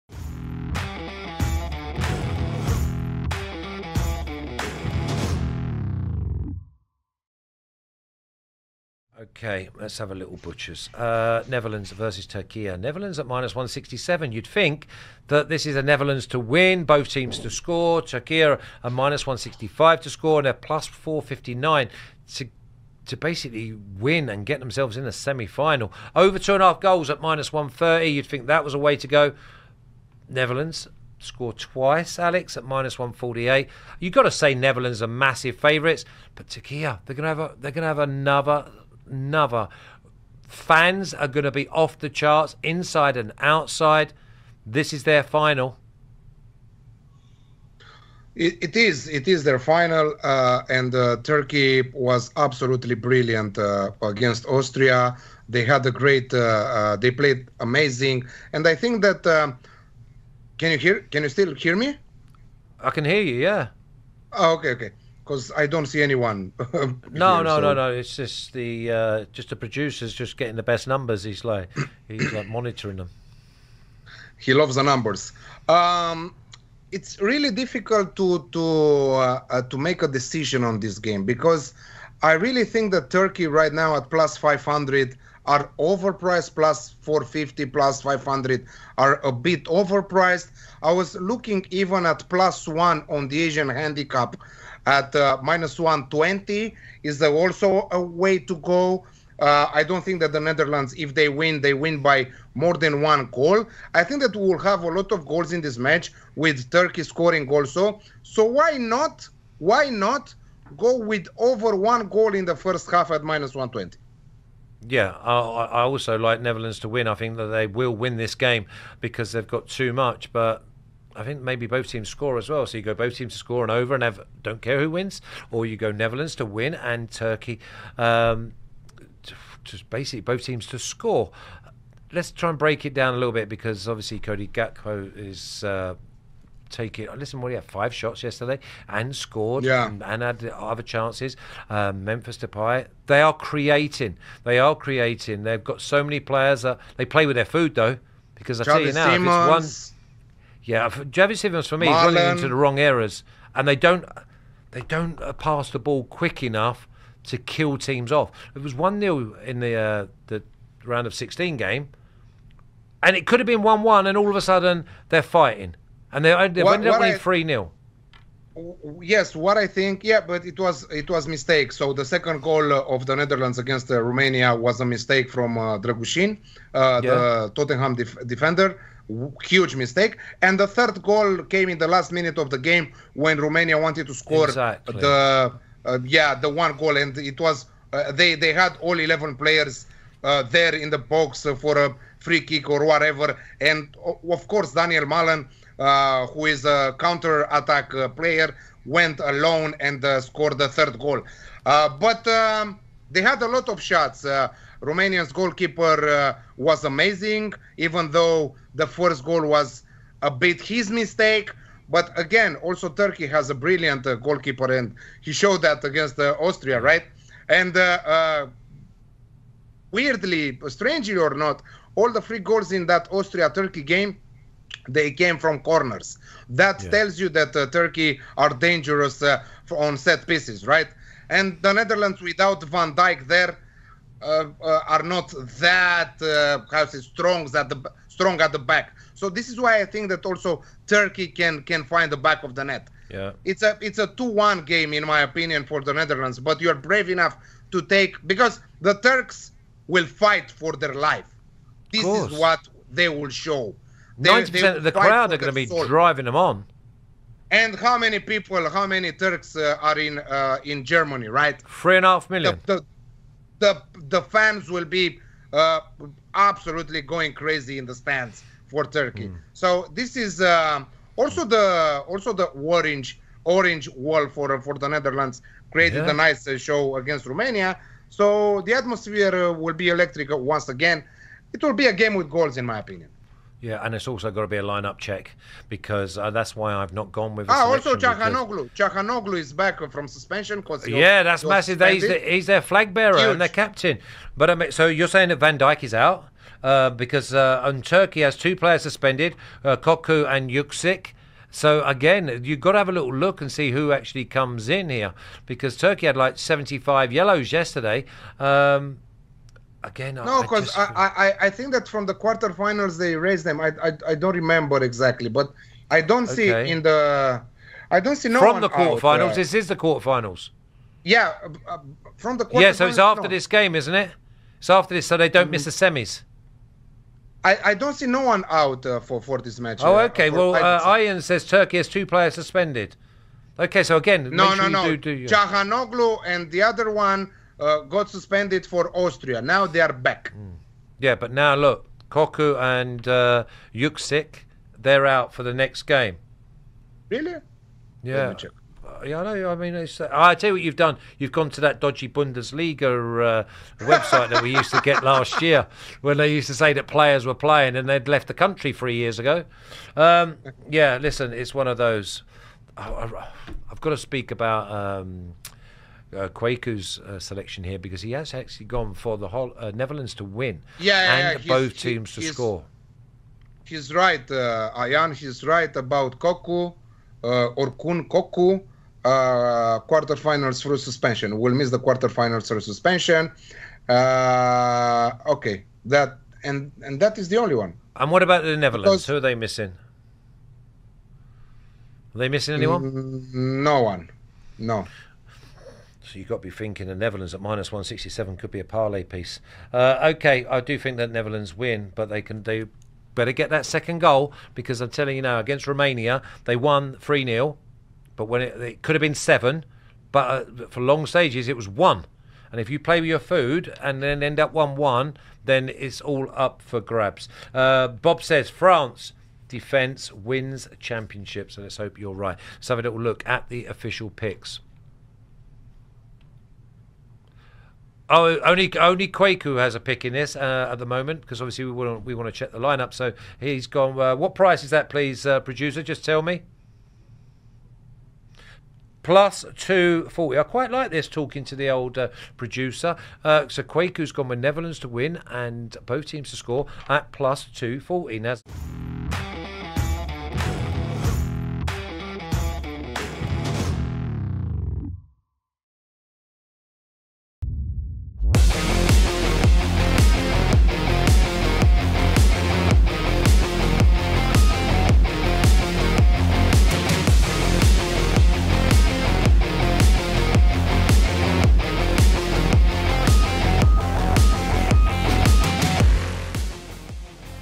Netherlands vs Turkiye | EURO 2024 Expert Predictions, Soccer Picks & Best Bets
Join our BetUS host, Gordon “Flash” Watson and soccer experts, Alex Campean and Kweku Afari, to discuss the best Euro 2024 odds available, and to share with you their hottest soccer predictions, free tips, and best soccer picks for the Euro 2024

Connect with our hosts and experts on X:
Flash: @Flash_37
Alex Campean: @AlexClassicTips
Kweku Afari: @Kweku_Afari